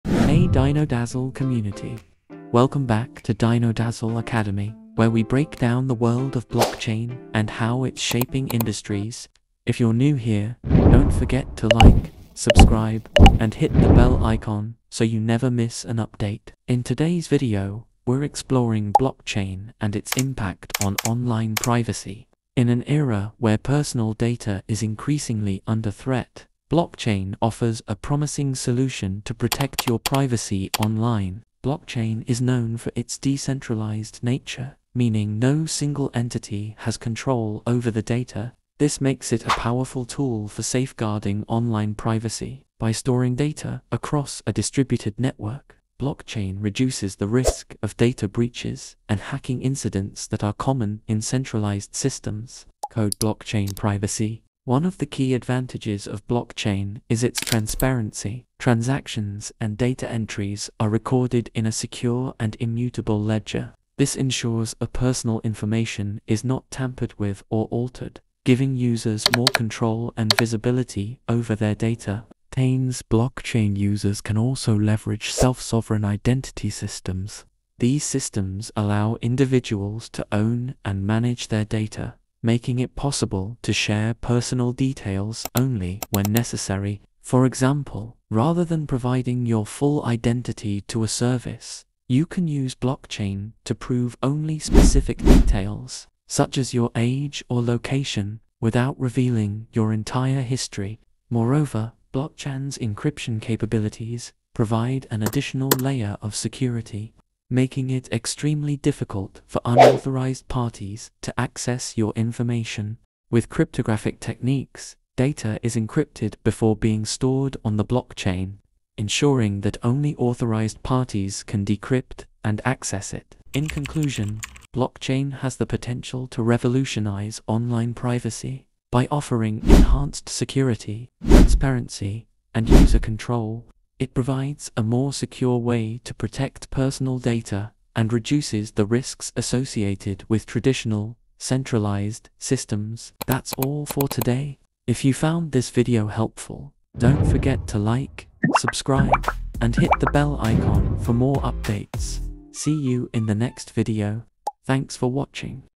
Blockchain and Its Impact on Online Privacy
Blockchain technology is revolutionizing the way we think about online privacy. By decentralizing data storage and transactions, blockchain minimizes the reliance on intermediaries, reducing vulnerabilities to hacking and data breaches. It empowers individuals to control their personal information through transparent and secure mechanisms like encryption and smart contracts. However, challenges like regulatory concerns and the potential for misuse remain. As blockchain evolves, it holds the potential to reshape online privacy standards across industries, from finance to healthcare.